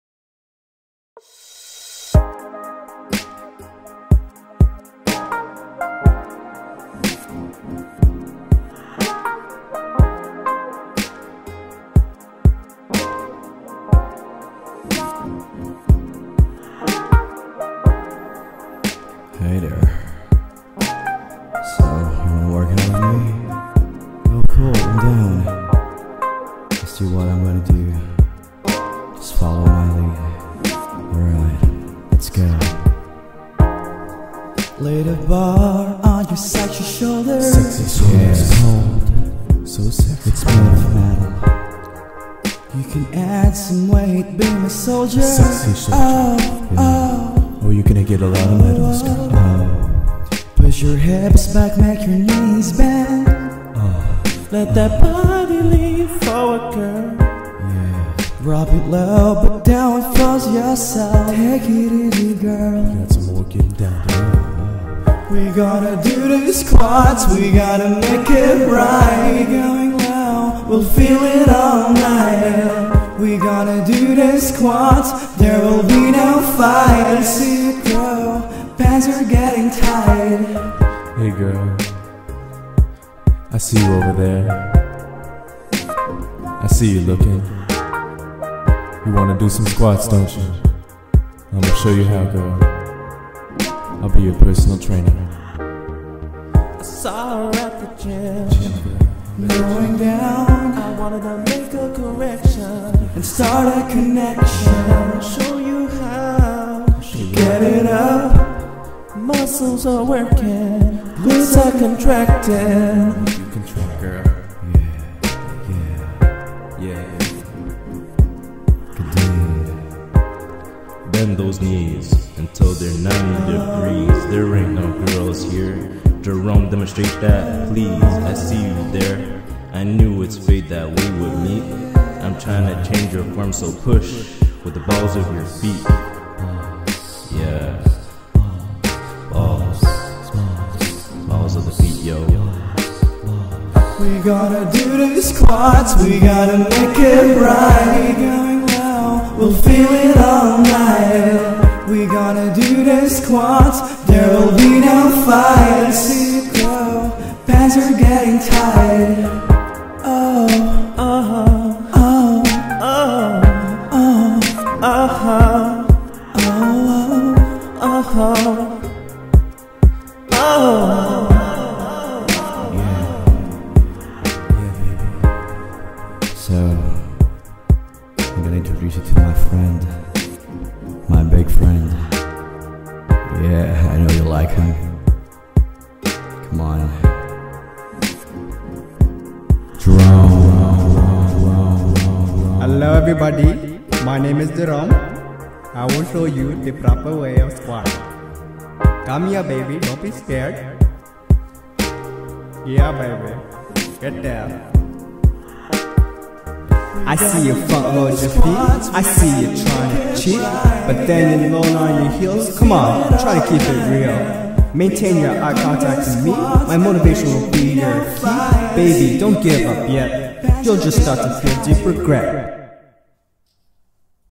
Squat bromance mix.
Mark and Isaiah feat. Jerome and Kadeen
a video for biomechanics class about the exercise squats.

forgot to double-check the texts, just realized that there's something wrong after an hour and a half of rendering. lol
aint nobody got time for reediting